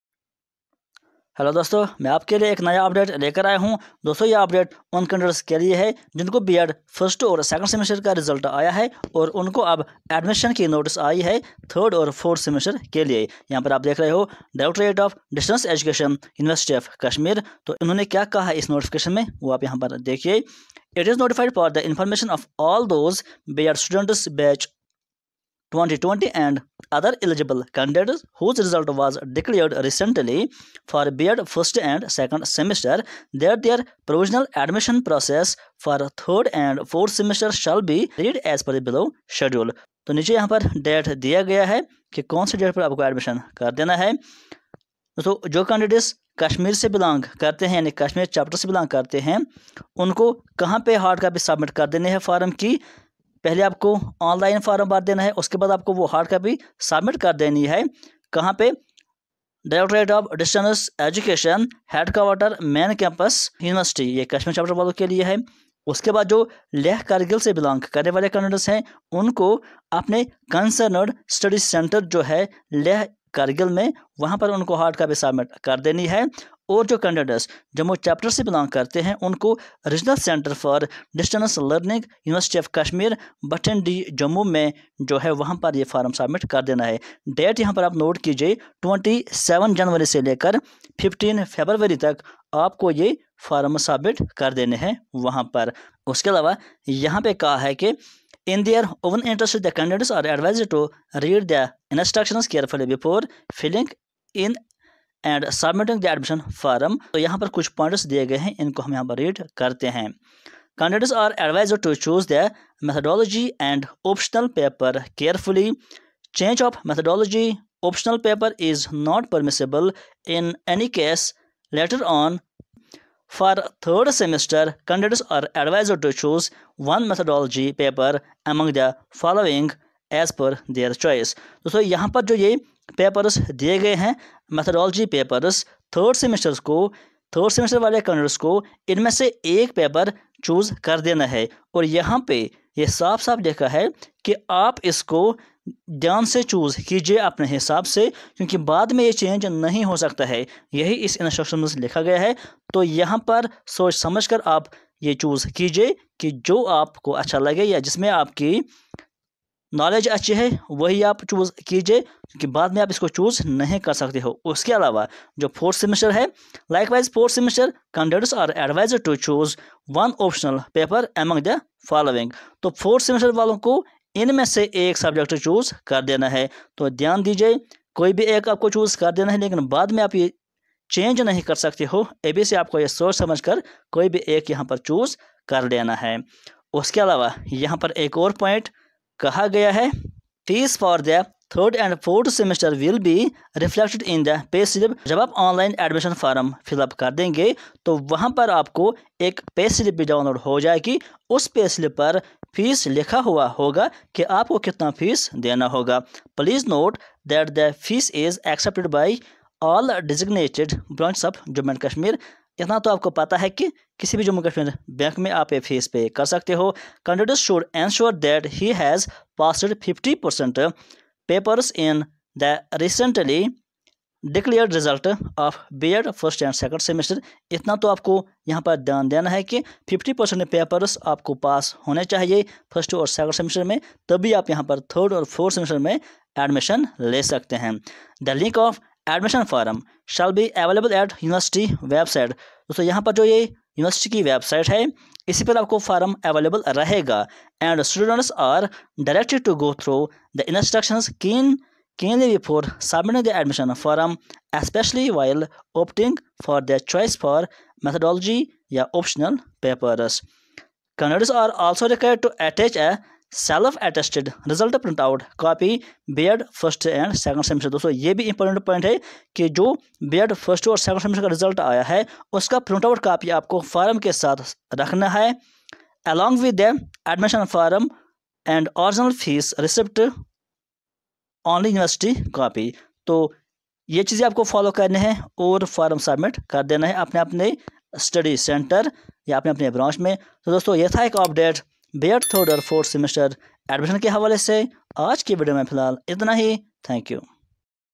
B. Ed Admission 3rd And 4th Semester. Important Points To Chose Optional Papers. Distance Education
In this channel we provide all notifications related to university of Kashmir and distance education.

javaidkhan.education